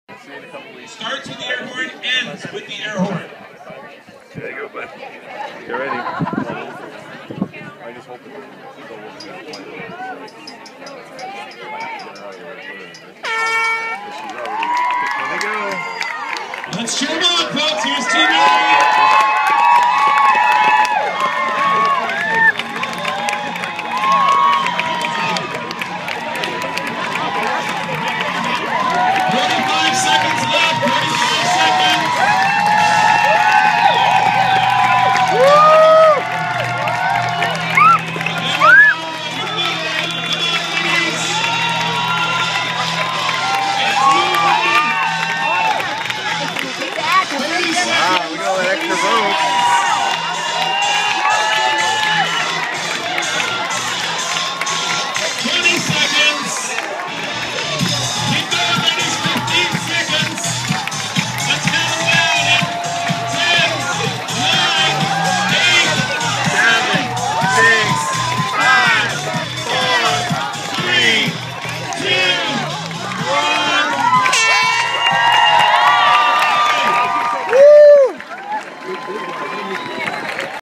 12 women pulling a train
My wife and 11 other women pulling a GO train 58' 10" for the barrie food bank and a chance to be in the Guinness Book of World Records.